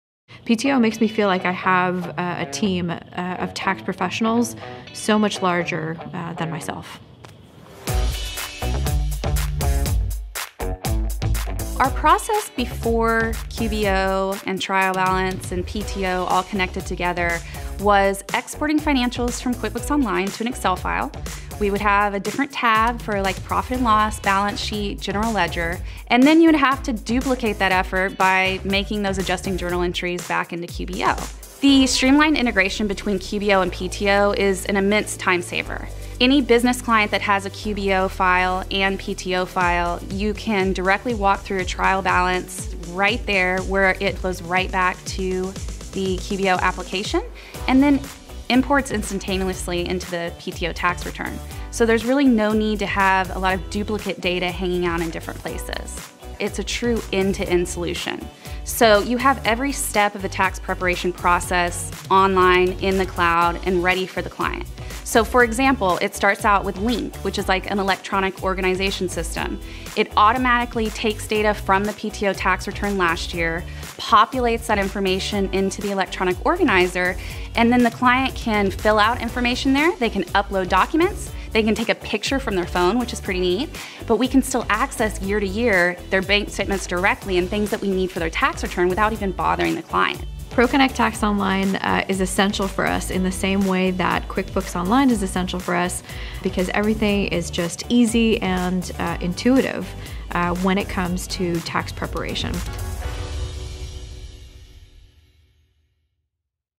Streamline Your Tax Prep with QuickBooks Online Accountant and ProConnect Tax Online
Working with QBOA and PTO is like having a virtual team of tax professionals right at your fingertips.

PTO makes me feel like I have a team of tax professionals so much larger than myself.

Our process before QBO and Trial Balance and PTO all connected together was exporting financials from QuickBooks Online to an Excel file. We would have a different tab for, like, profit and loss, balance sheet, general ledger, and then you would have to duplicate that effort by making those adjusting journal entries back into QBO.

The streamlined integration between QBO and PTO is an immense timesaver. Any business client that has a QBO file and PTO file, you can directly walk through a Trial Balance right there, where it goes right back to the QBO application and then imports instantaneously into the PTO tax return.

So there's really no need to have a lot of duplicate data hanging on in different places. It's a true end-to-end solution. So you have every step of a tax preparation process online, in the cloud, and ready for the client.

So for example, it starts out with Link, which is like an electronic organization system. It automatically takes data from the PTO tax return last year, populates that information into the electronic organizer, and then the client can fill out information there, they can upload documents, they can take a picture from their phone, which is pretty neat.

But we can still access year to year their bank statements directly and things that we need for their tax return without even bothering the client.

ProConnect Tax Online is essential for us in the same way that QuickBooks Online is essential for us because everything is just easy and intuitive when it comes to tax preparation.